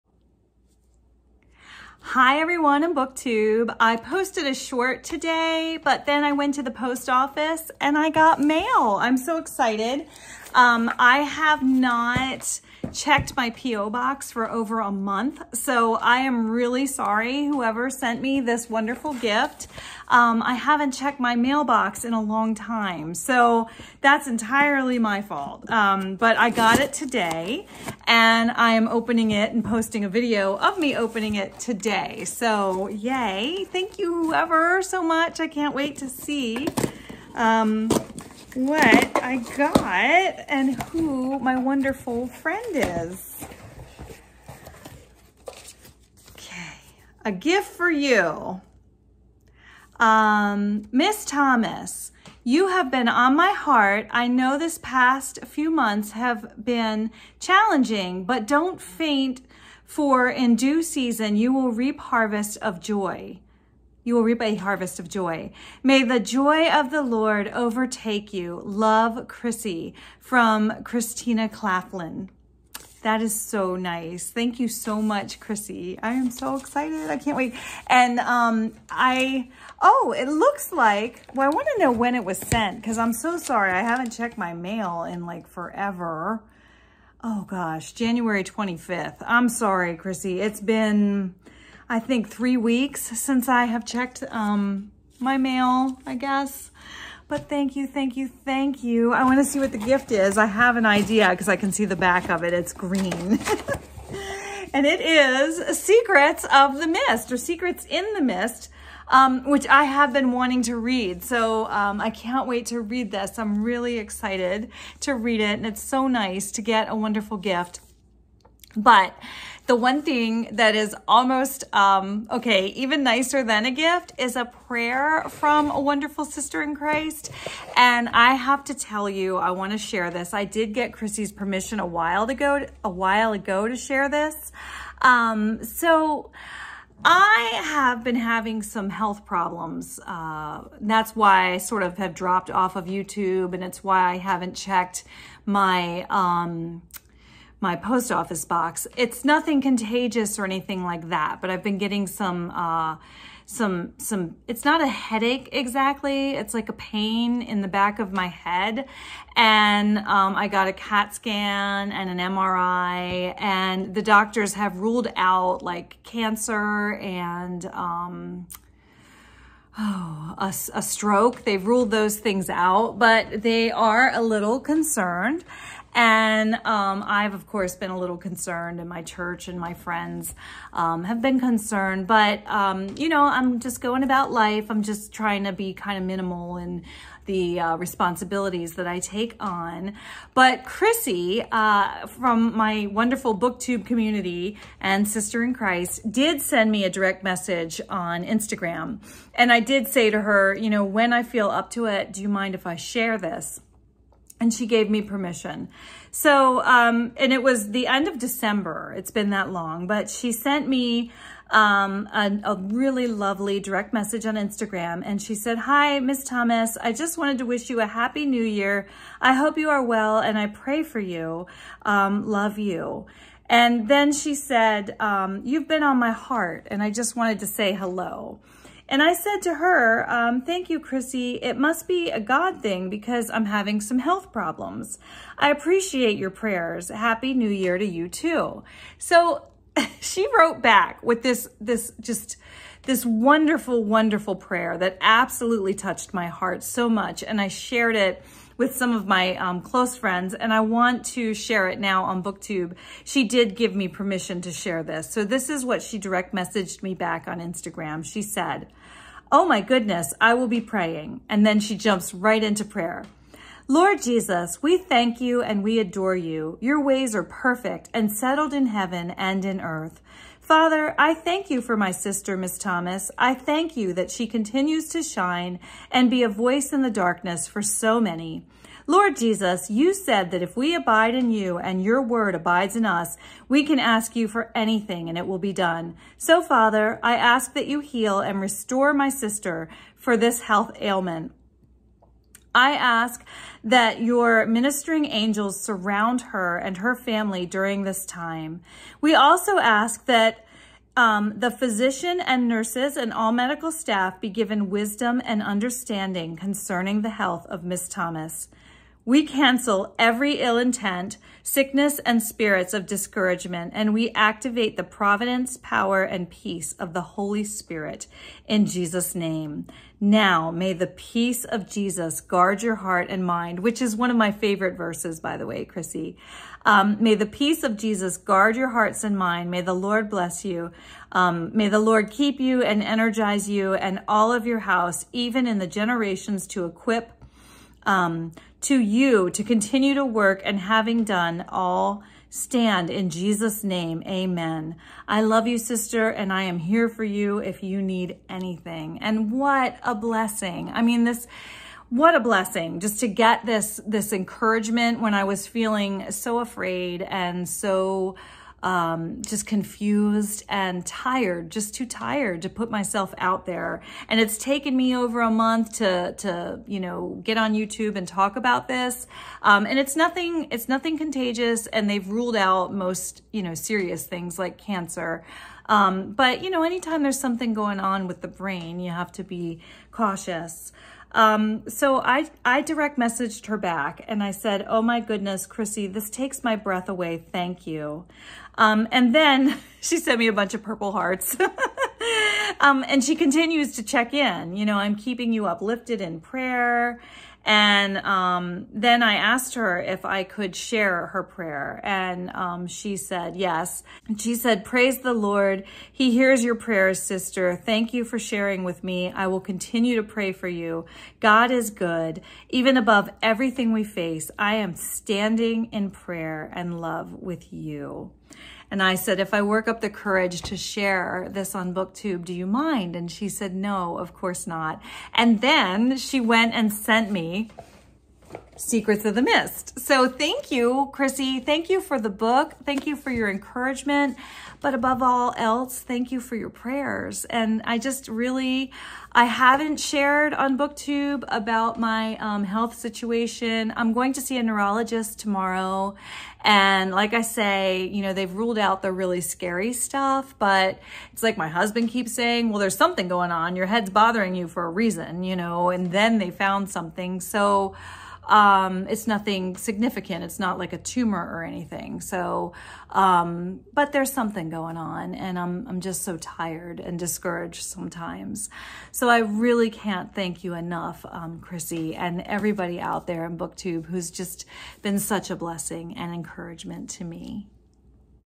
BookMail from Christian BookTube
Book Mail!  Find out about a wonderful surprise as I unbox book mail and let me share a beautiful prayer from my beautiful sister in Christ.

I have a bookish wishlist on Amazon now too.